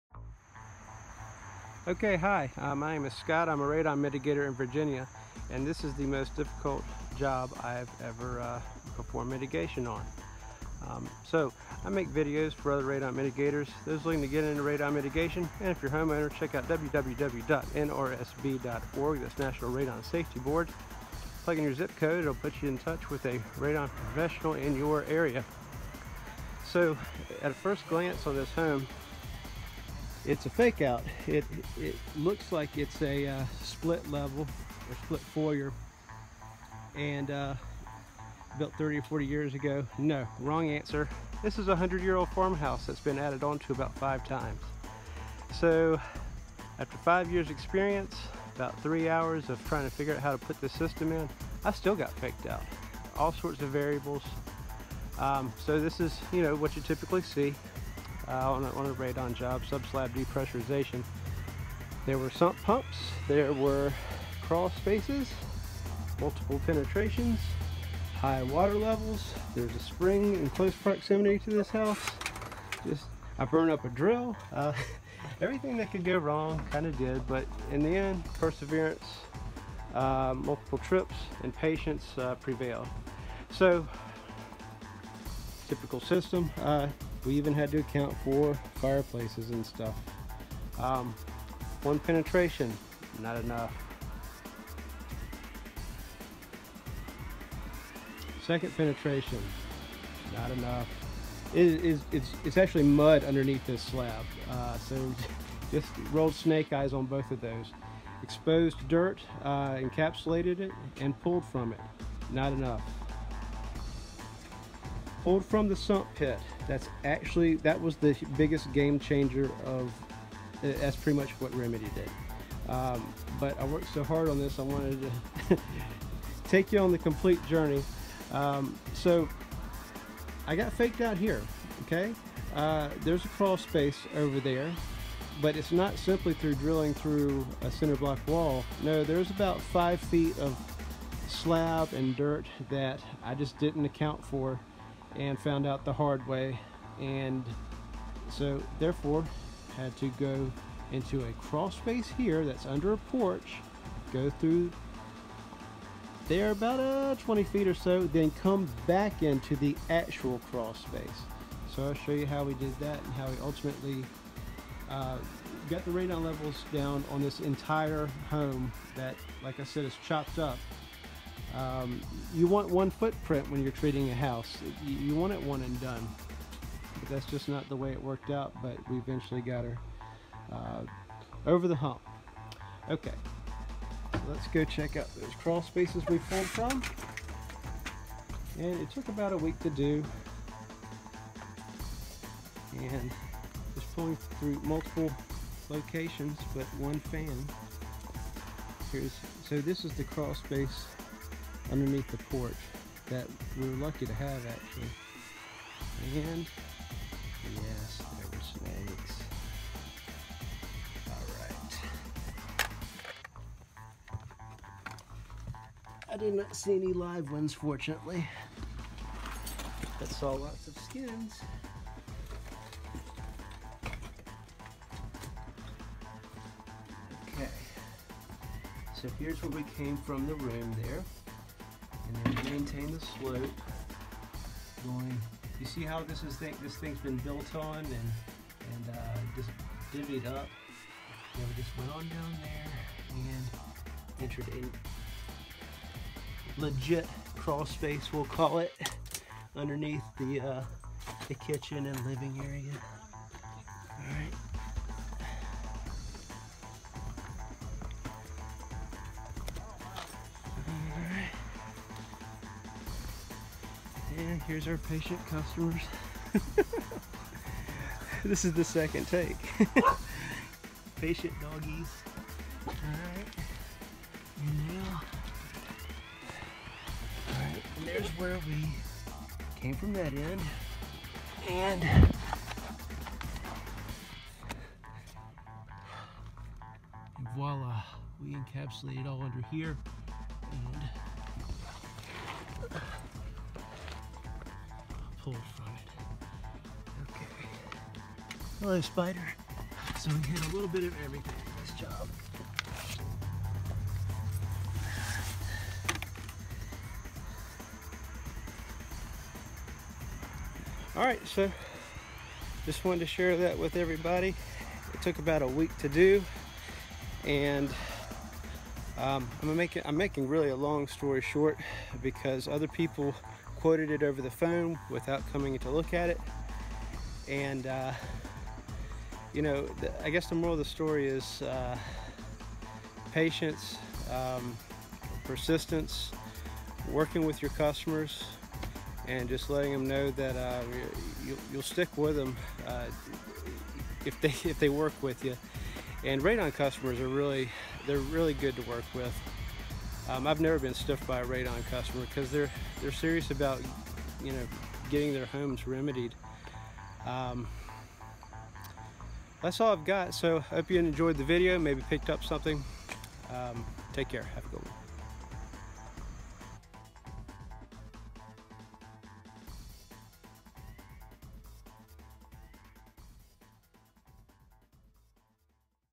The Most Difficult Radon Mitigation Job
This video is about the most difficult radon mitigation/ abatement job I have encountered
during my 5 years in the field of radon measurement and mitigation. It involved the suction of radon from multiple locations in the home and yet tying them to just one fan.